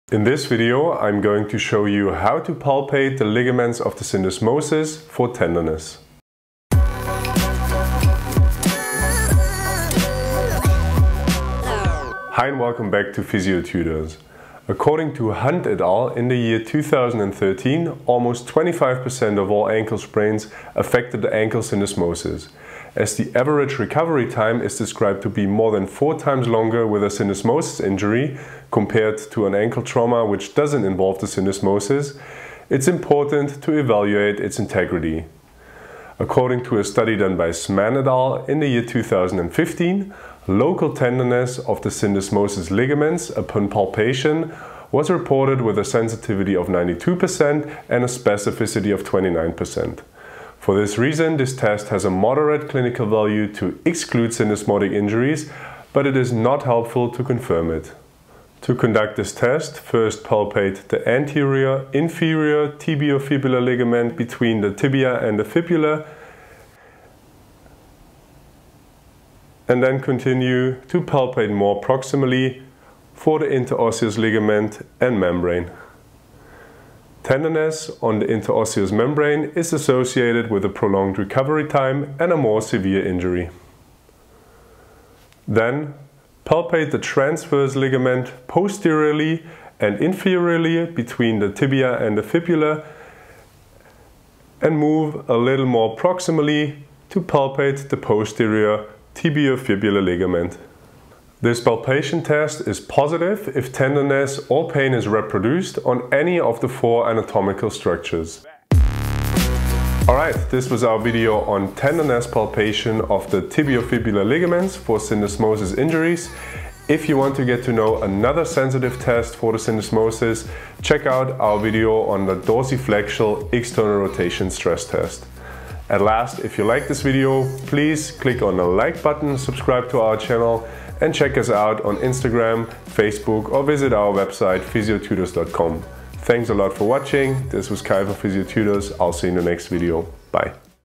Syndesmosis Ligament Tenderness Palpation | Syndesmosis Injury
The content is intended as educational content for health care professionals and students. If you are a patient, seek care of a health care professional. Palpating the ligaments of the ankle syndesmosis for tenderness can help indentify concordant syndesmosis injury after ankle inversion trauma.